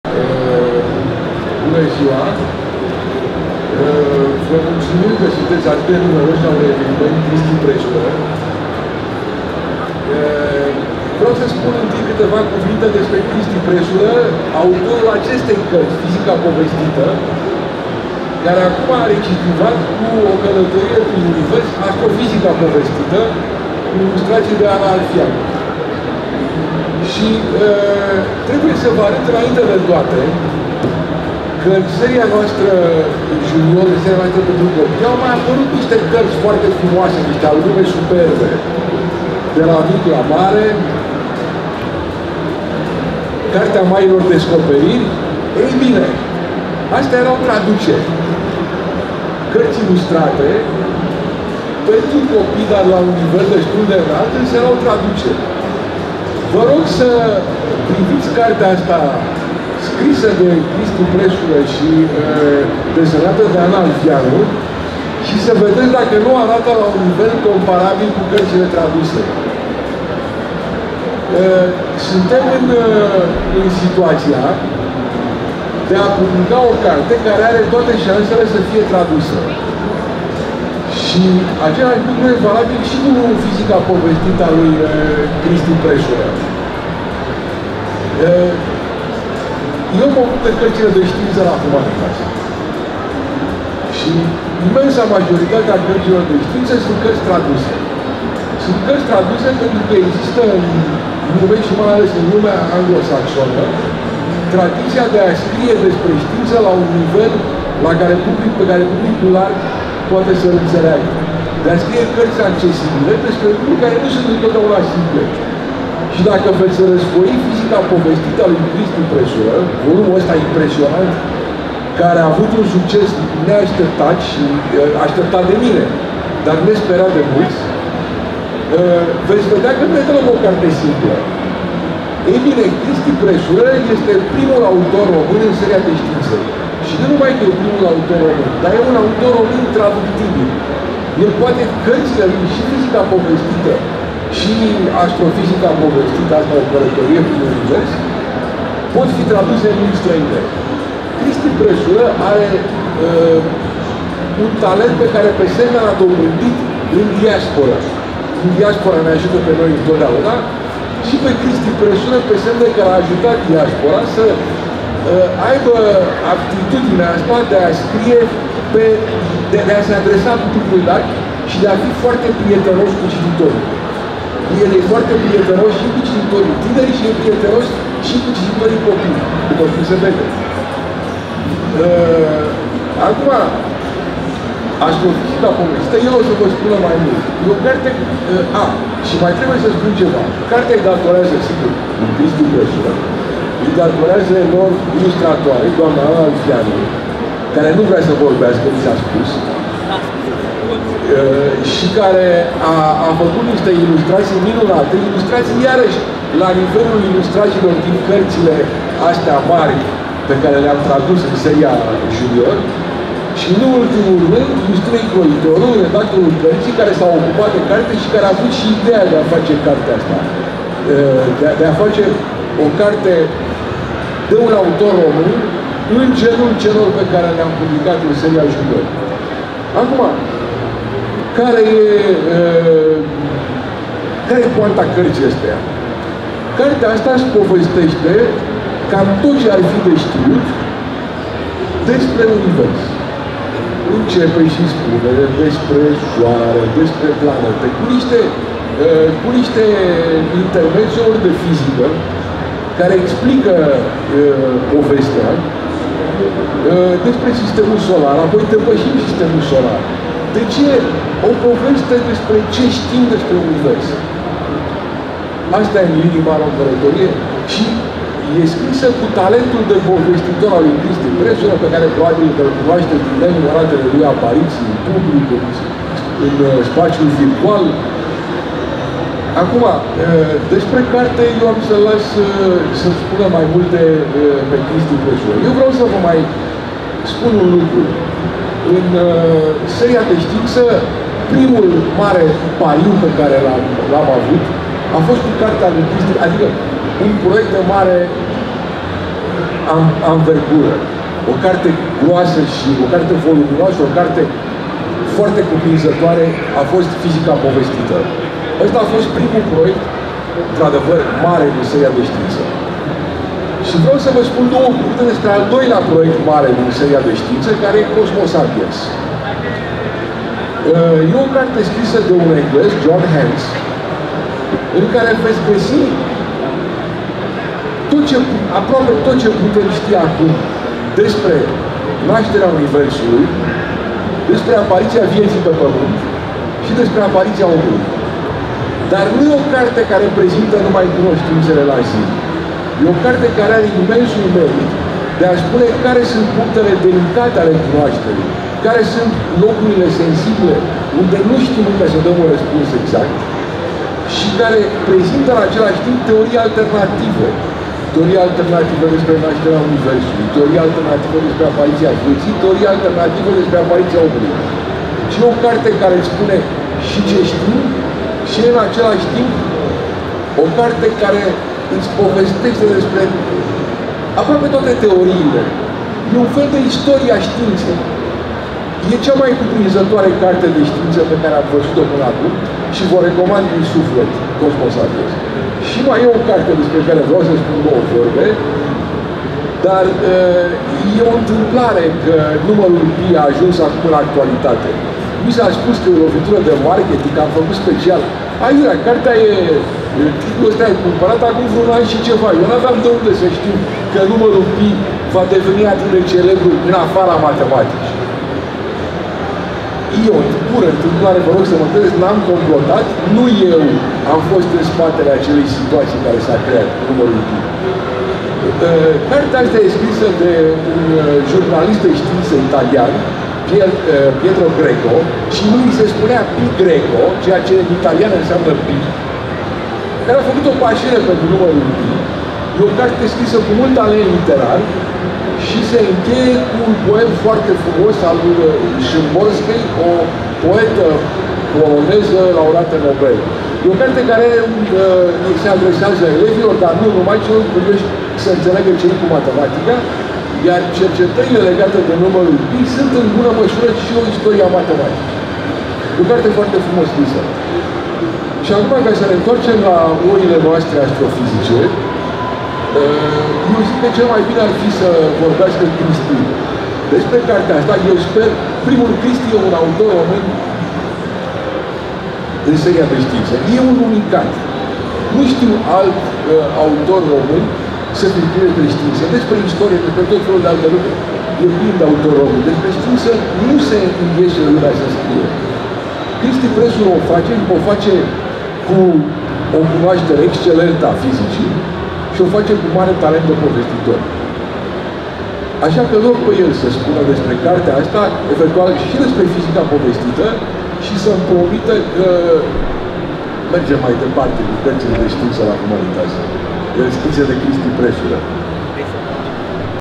(01) Vlad Zografi: Lansare carte - O călătorie prin Univers. Astrofizica povestită, Cristian Presură
În data de 23 noiembrie 2019 a avut loc lansarea de carte "O călătorie prin Univers. Astrofizica povestită" de Cristian Presură, cu ilustrații de Ana Alfianu, Colecția Humanitas Junior în cadrul târgului Gaudeamus 2019.

Vorbitori: Cristian Presură, Vlad Zografi.